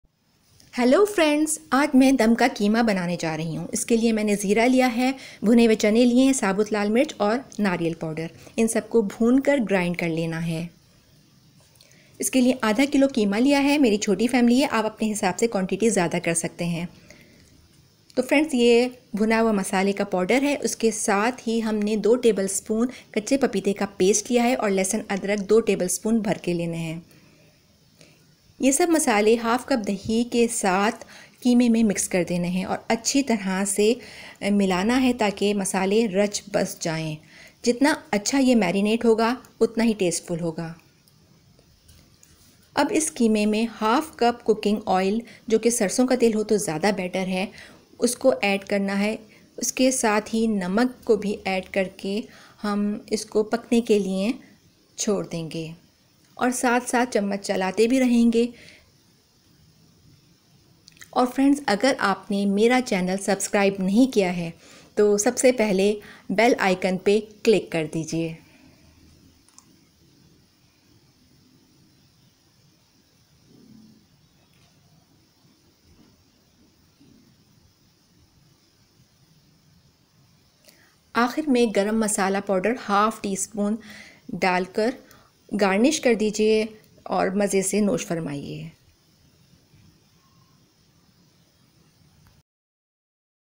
Dam ka Qeema .
This is a simplest way of cooking dam ka Qeema .
Ingredients
Half kg mince (Beef, Mutton )
Half cup yoghurt
Half cup oil ( Mustard oil recommend)
1 TB Cumin seeds , zeera
4 whole button red chilli
2TB bhunay chanay
1 TB coconut pd
2TB raw papaya (kacha papeeta)
Method
 in a pan put Zeera ,Nariyal pd , bhunay chanay and whole red chilli and make them dry roast , and after that grind them , make fine powder
In half kg Qeema add , raw papaya, roasted masala pd , ginger garlic , yoghurt and let it marinate for at least 2 _3 hours
Take have cup oil in a cooking pot, add marinated mince ,cook for 25 minutes.
garnish with onion rings, ginger , Lemon and mint .
          Regards.